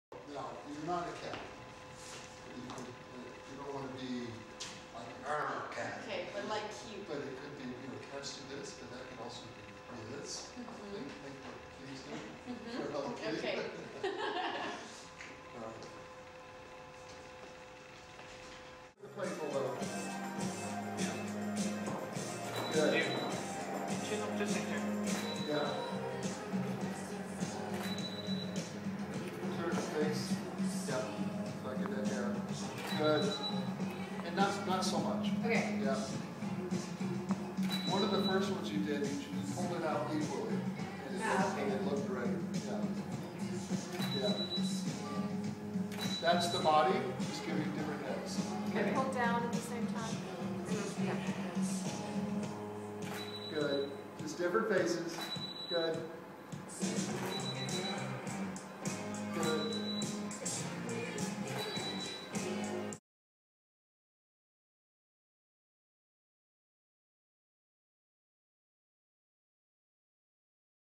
FASHION PHOTOG RICHARD WARREN LINGERIE PHOTO SHOOT
Fashion Photographer Richard Warren shooting new lingerie campaign